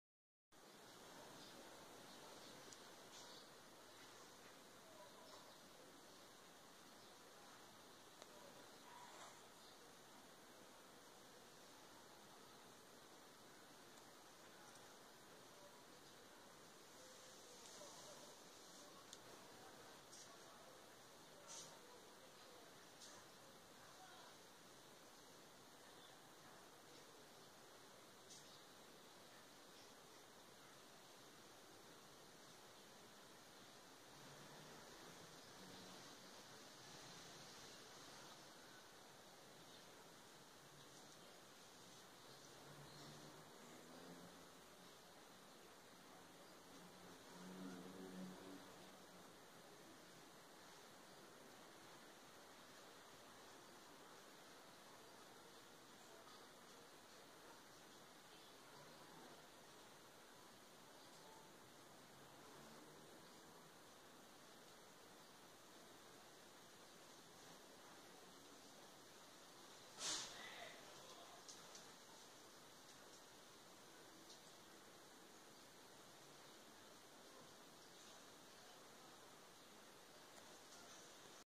Un grillito vive en mi casa :)
Me encanta el ruidito que hacen los grillitos en las noches. Todavía no se escucha.